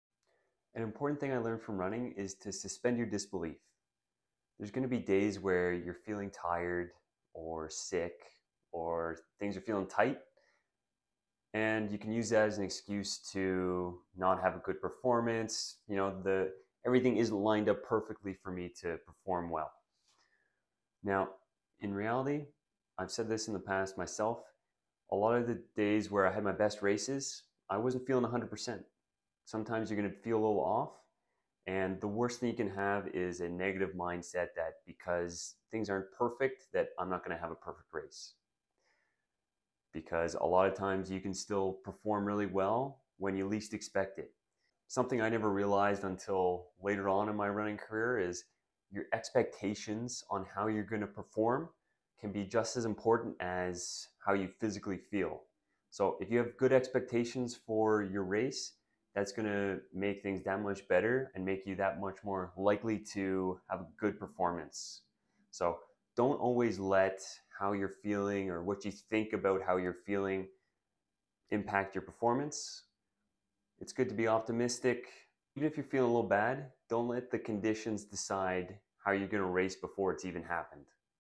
Running: Remaining Optimistic
The key to success in running regardless of the circumstances, is remaining optimistic about your race. your body gives you feedback on how everything feels but sometimes we listen to it too much. The most important guide to performance is your perceptions on how you will perform.

Perceive that you will have a bad race and you will make your thoughts reality. Running is hard enough, go into a race with an open mind and prove despite how you may feel, you can still have an excellent performance. I've personally found most of my break through races were on day where I didn't feel 100%.

Share your race experiences in the comments!